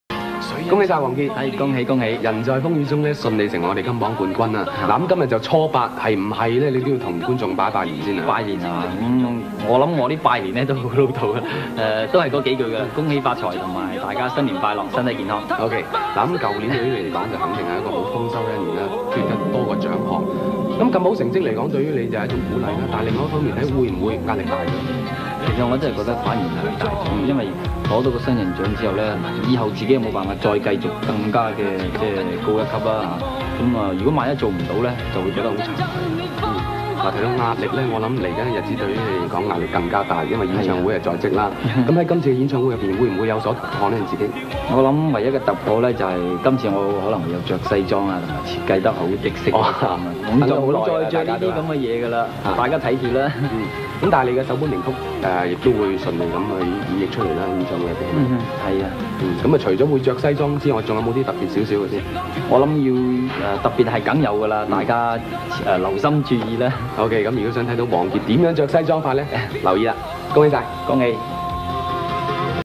【傑回顧-1990】 王傑 - 大年初八拜年 + 訪問（附上字幕）
大年初八，傑迷們快接收傑哥自嘲很老土的新年祝福吧[嘻嘻]